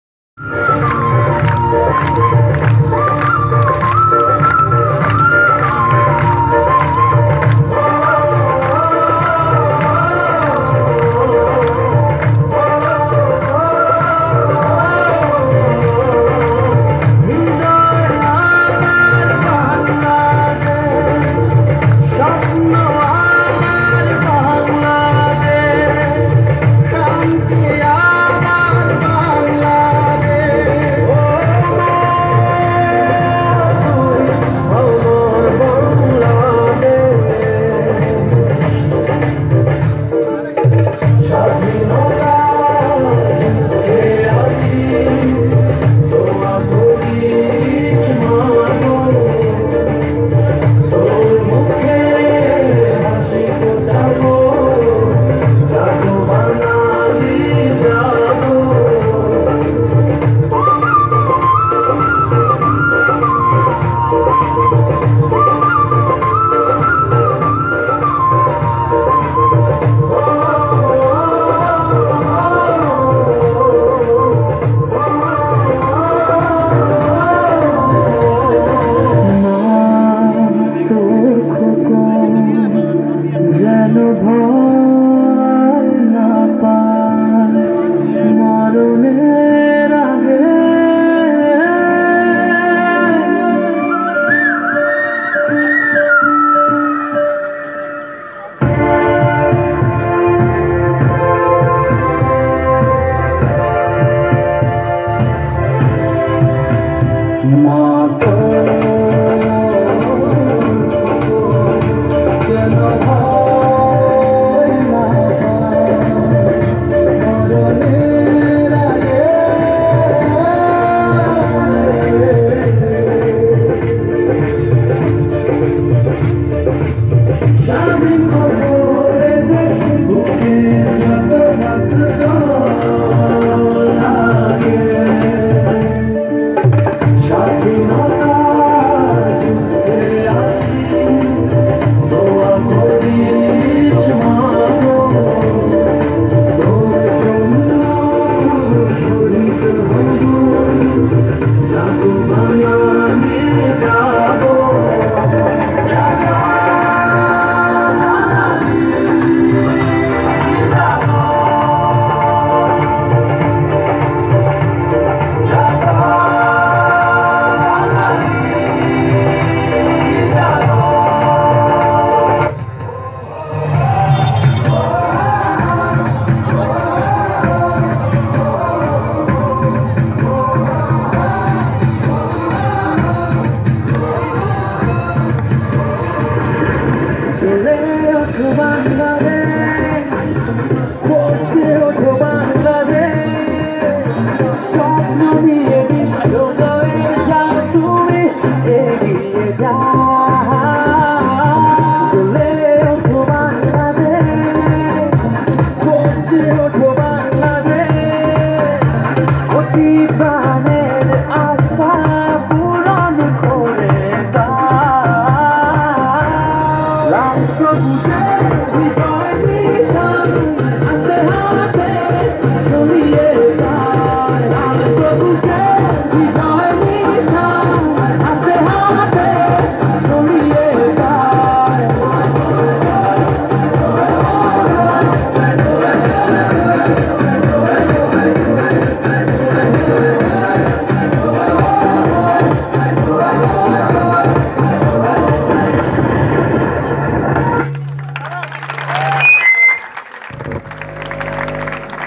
Dance at opening ceremony of inter faculty cricket tournament -2019, PSTU.
Dance at opening ceremony of inter faculty cricket tournament -2019, Patuakhali Science and Technology University.